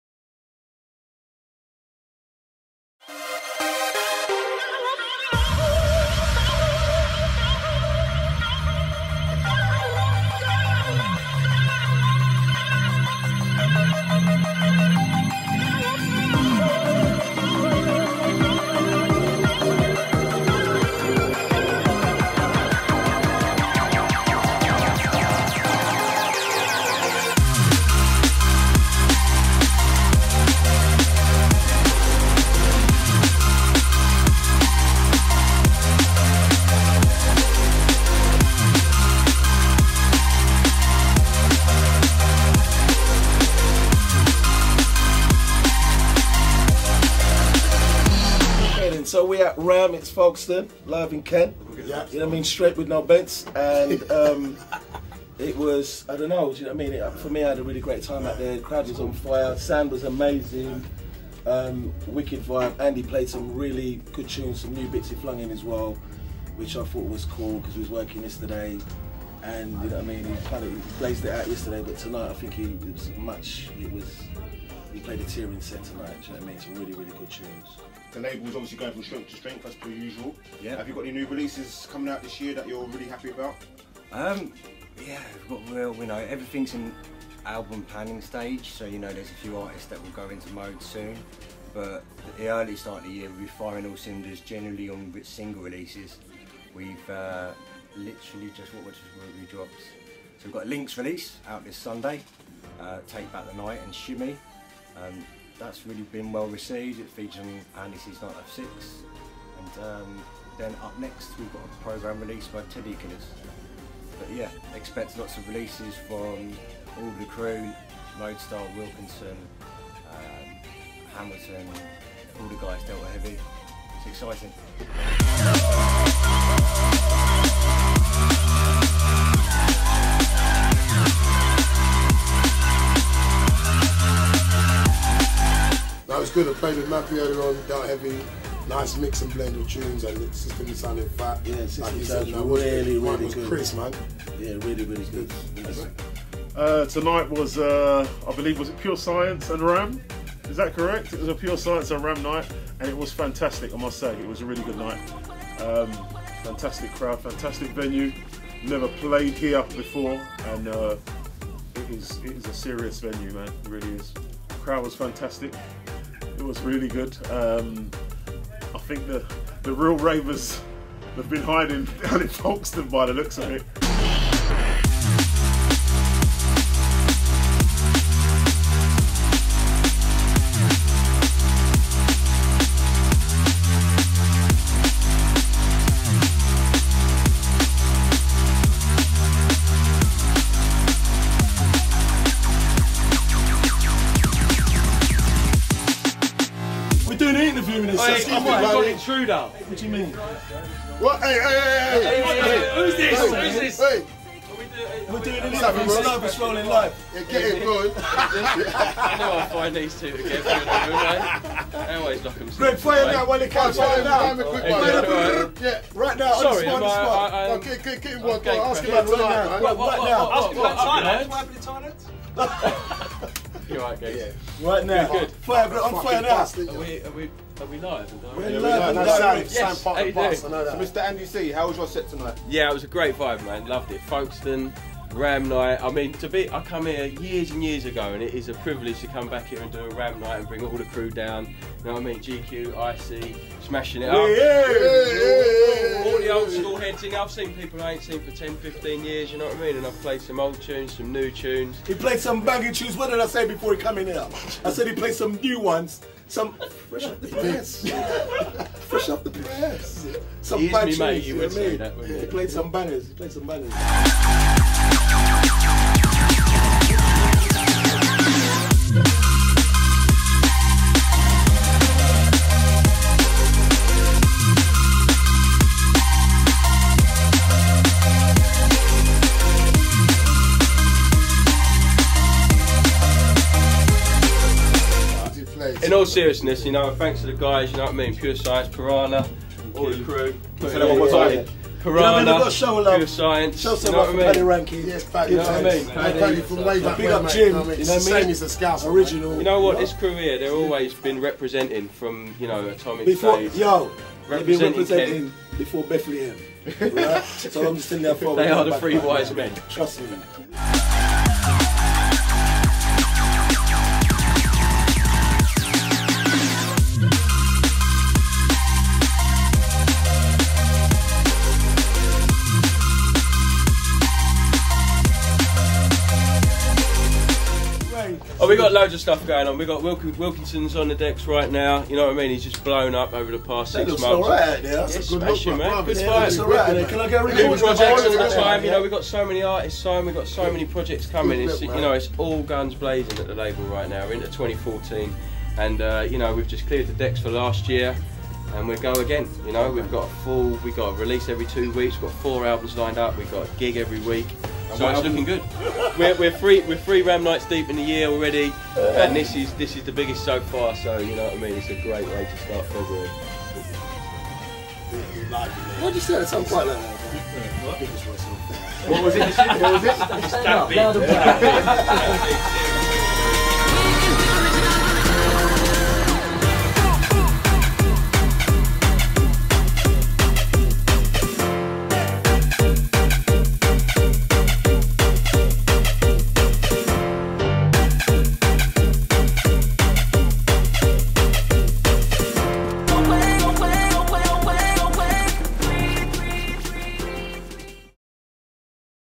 RAM Kent V Pure Science Highlights from Leas Cliff Hall 1.02.2012
Pure Science Invited Andy C And Ram to get their boys down to Folkestone for a monster all nighter in a historic venue

Feat interviews with all the artists and the best bits of the action !